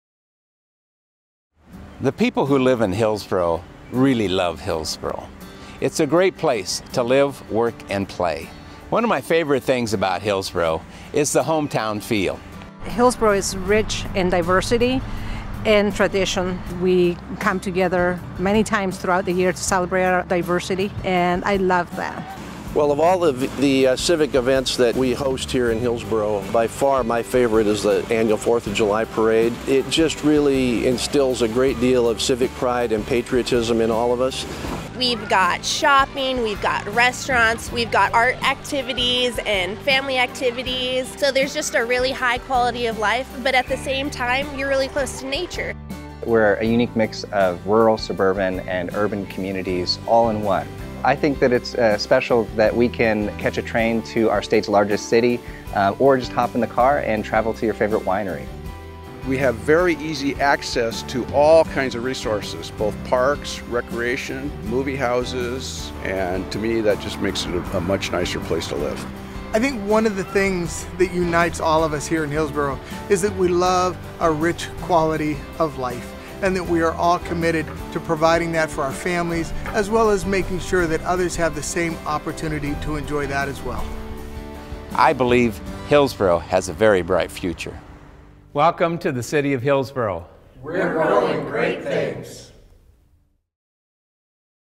Hillsboro, Oregon City Council Welcome 2015
Mayor Jerry Willey and City Councilors share what makes Hillsboro a special place. Hear about our rich diversity and tradition, civic events like the 4th of July Parade, numerous family activities, and countless resources that make Hillsboro, Oregon so special.